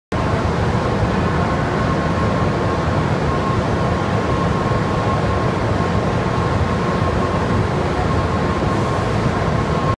ALTEK Side Mounted Electromagnetic Stirrer (EMS) Gentle Stirring Mode
ALTEKs Low Energy, Air Cooled Electromagnetic Stirrers (EMS) are very versatile and can be installed onto the side, back or bottom of almost any furnace in the industry.  This can be on new furnaces or retrofitting onto existing furnaces.

Installed in early 2015, this video showcases a Side Mounted installation of Altek's TYPE 400 Electromagnetic Stirrer.  On a low power mode typically used for the holding stage of a furnace cycle, this gentle circulation maintains chemical and temperature homogeneity in the furnace bath whilst also keeping dross formation and power consumption down to a minimum.